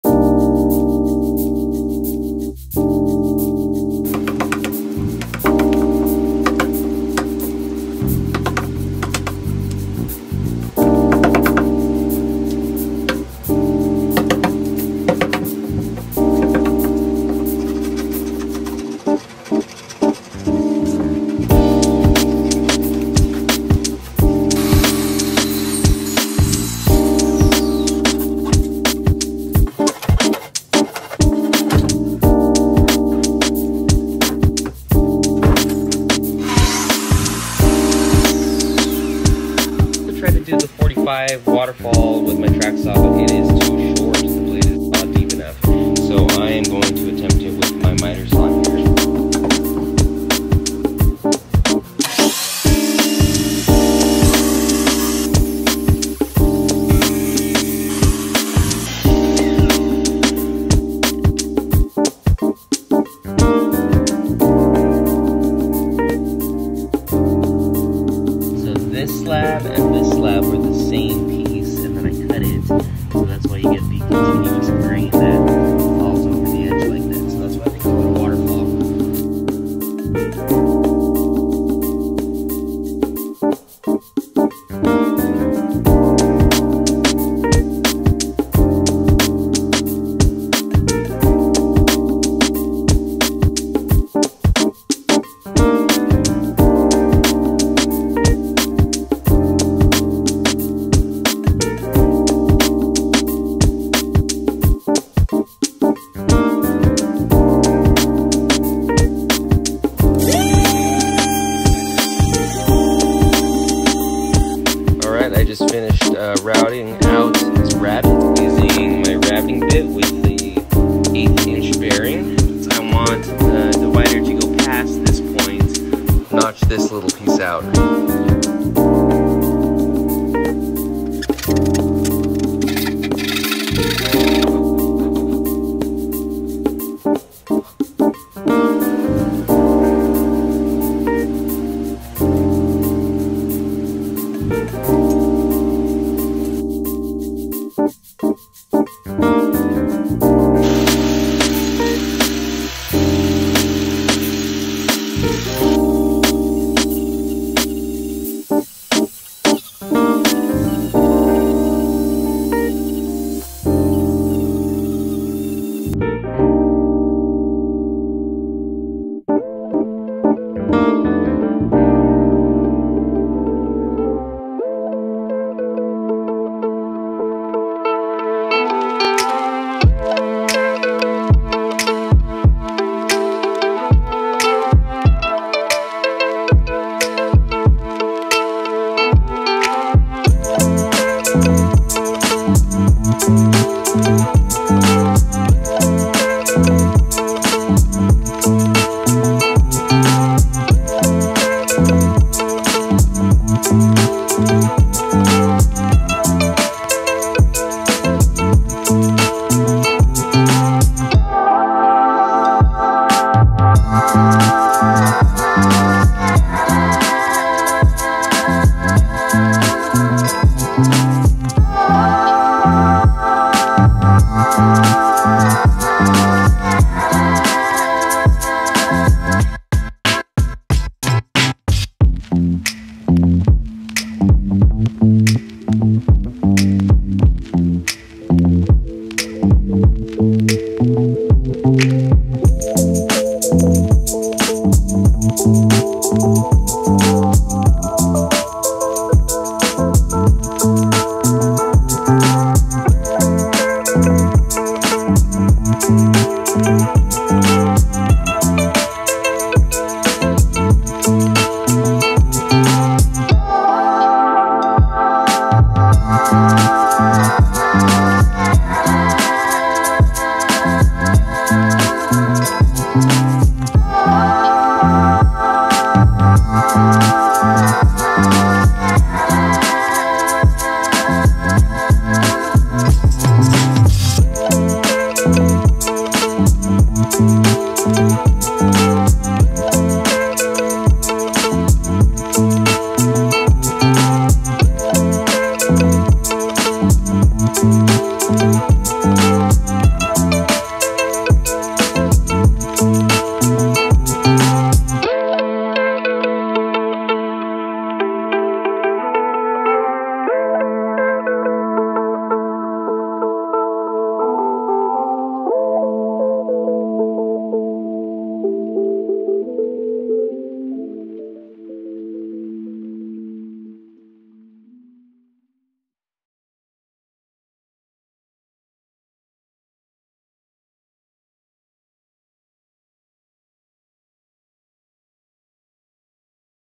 Live Edge Waterfall Console Table with Sliding Epoxy Doors | Ep. 8
Since this was going to be a live edge piece, I began by stripping off the bark and breaking down the slab into smaller more manageable pieces. I did this using my Makita track saw. I tried using my track saw to do the 45 degree cuts needed for the waterfall edge, but the blade depth was too shallow, so I ended up using my sliding miter saw, which worked out great!

Once the 45 degree miters were cut, I began routing out the tracks for the sliding doors as well as the dados for the dividers. After the dados and tracks were cut, I rabbeted some divider pieces and glued everything in place.

Using my kutzall shaping disc, I blended the live edges of the main frame to make it look seamless. I wanted to make the three sliding epoxy doors to be made out of one piece so it would have continuous grain across all three of them, I didn't have any more live edge pieces long enough to do this, so I ended up using the shaping disc to carve a faux live edge. I then built a melamine mold and poured some Superclear Deep Pour epoxy with a transparent black tint.

I then rabbeted and chiseled the back of the table to receive a backer board and glued that in place. The entire piece was finished with Rubio Monocoat Oil Plus 2C in "Pure". I really liked how this came out and it was the first piece I have made using only joinery and no fasteners!